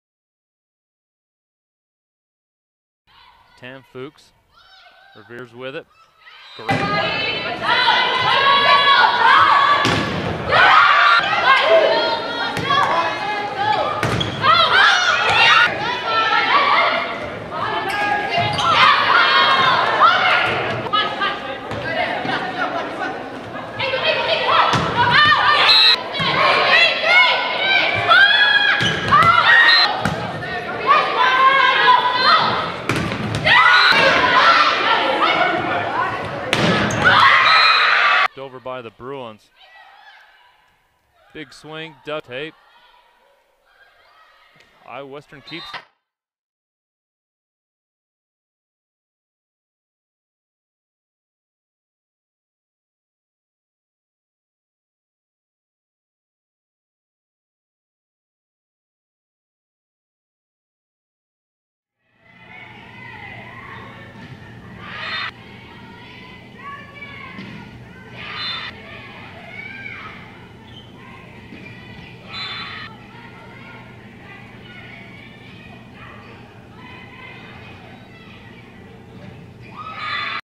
Imelda Aziabou Volleyball Highlight 2017
Imelda Aziabou #10 recruiting video
6'1" OH/RS
Current school : Iowa Western Community College